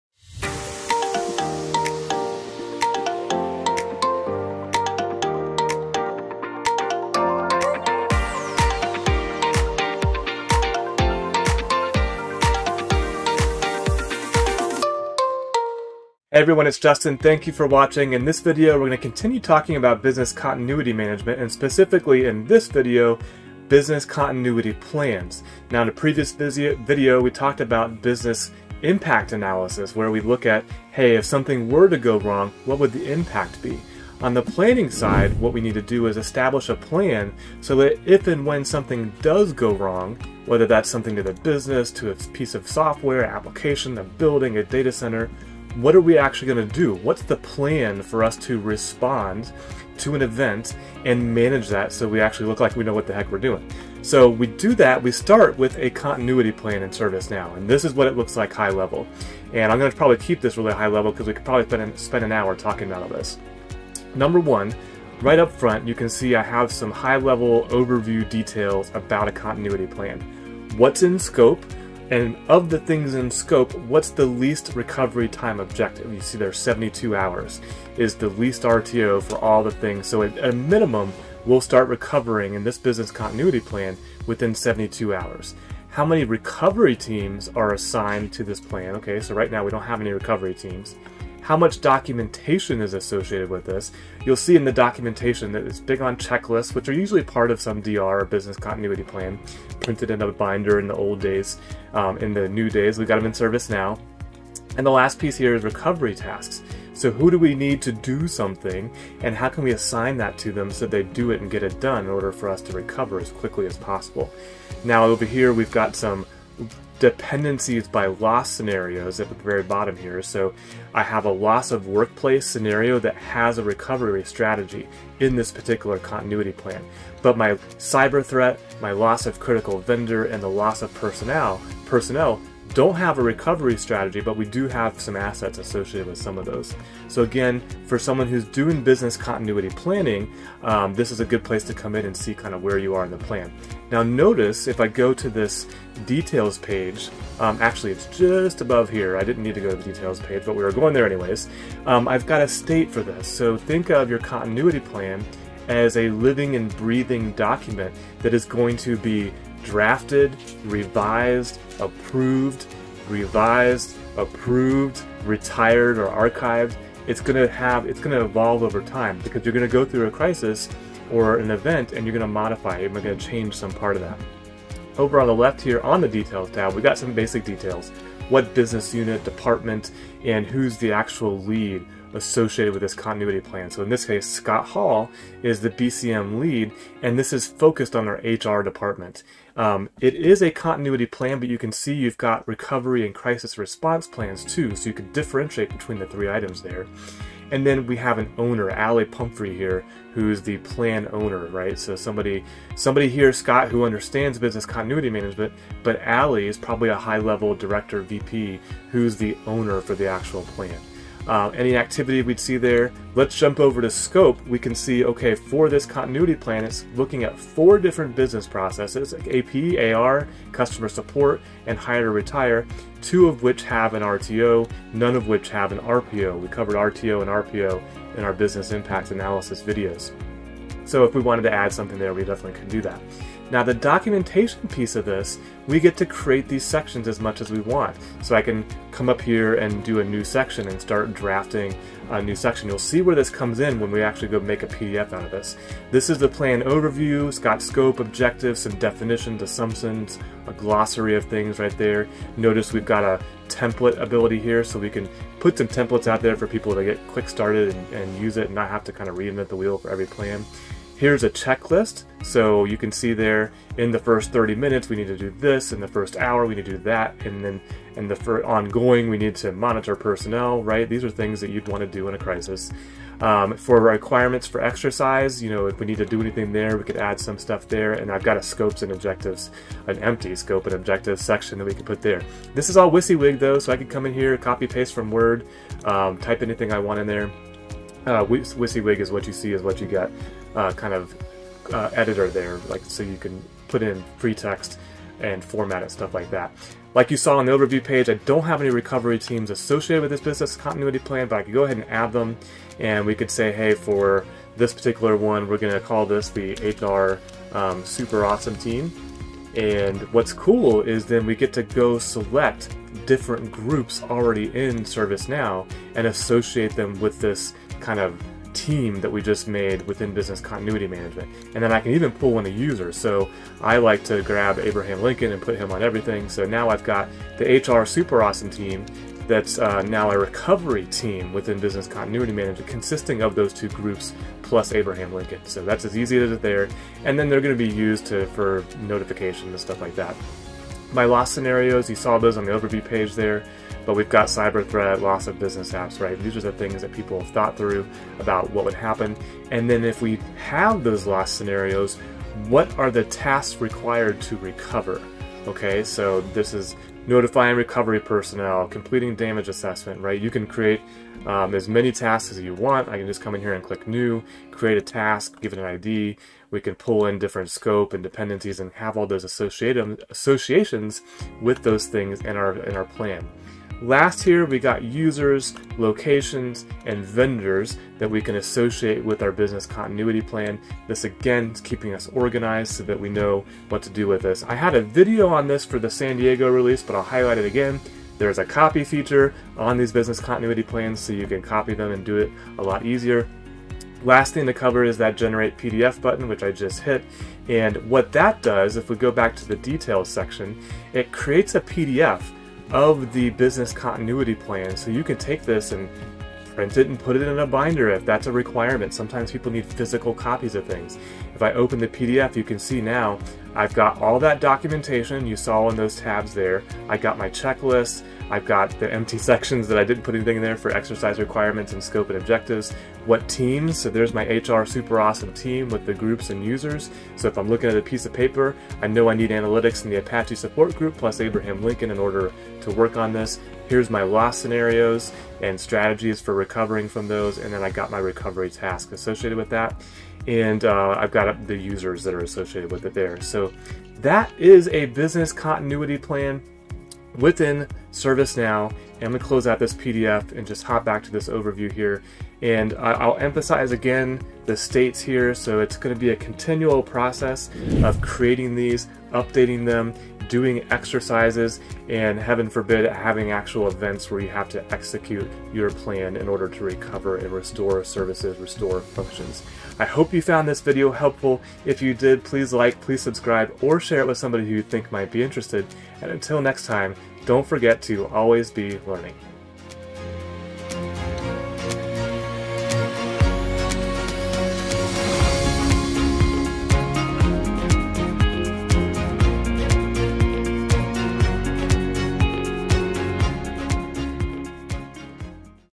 Business Continuity Management Continuity Plan
In this video we review what makes up a Business Continuity Plan in the Business Continuity Management application in ServiceNow. Complete with their own workflow to manage creating, approving and using these plans, they also include all of the pertinent details you might need when planing a response to an impacting event or crisis that impacts your organization.